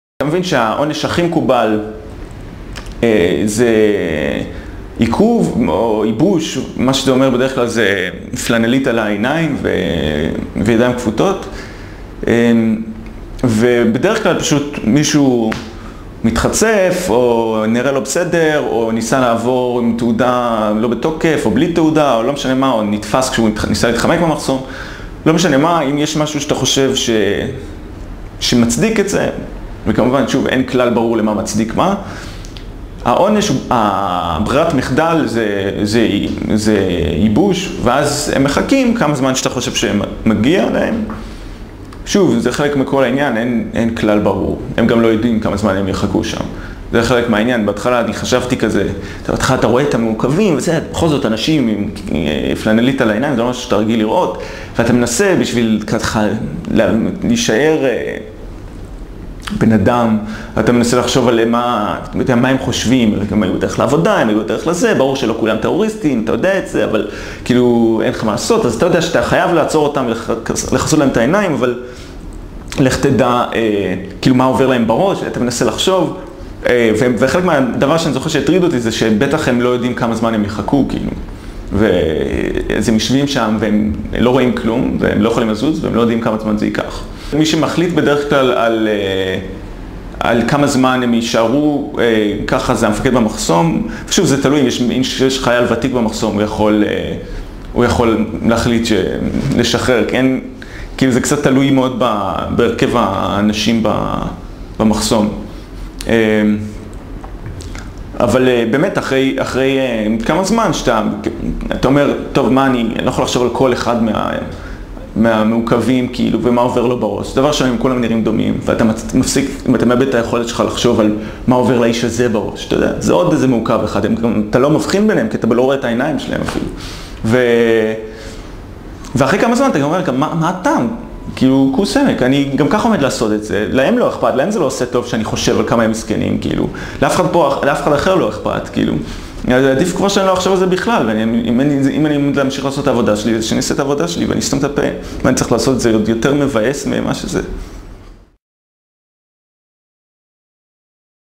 The standard punishment
The default punishment for a detainee is "drying out." But there are no clear rules for this procedure.